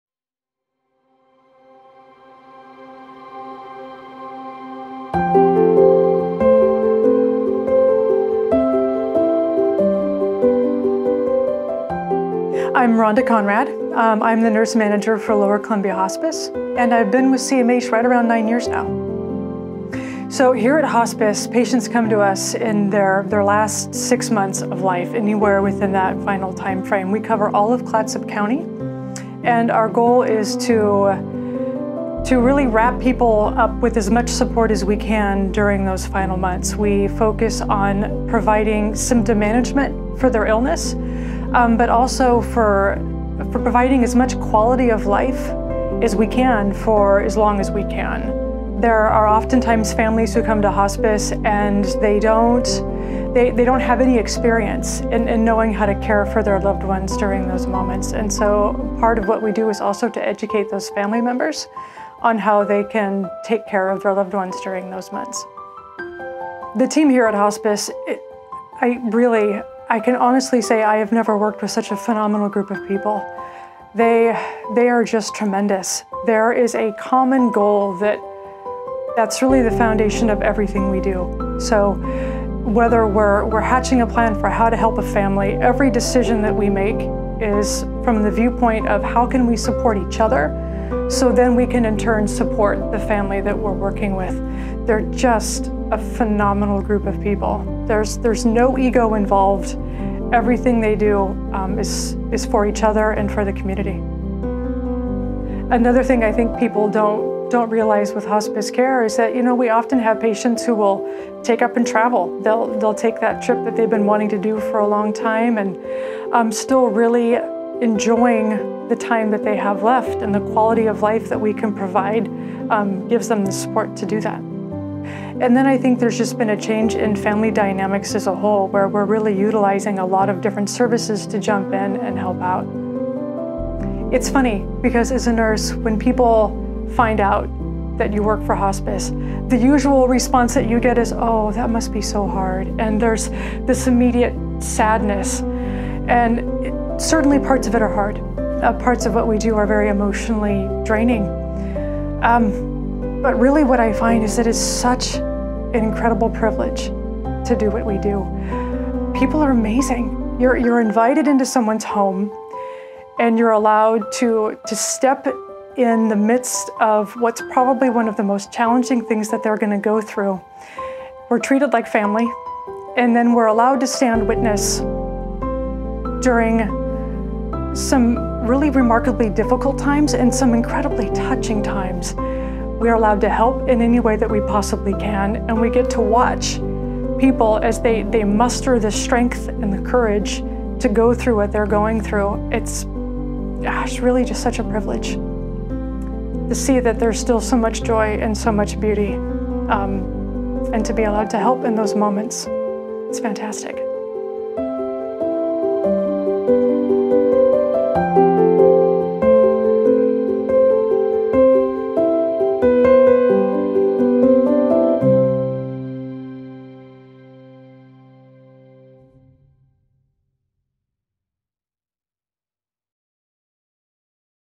Rhonda Conrad | Lower Columbia Hospice Manager
Rhonda Conrad, Manager of Lower Columbia Hospice, shares her insights about how hospice can help patients and about the incredible team of caregivers that provide hospice care.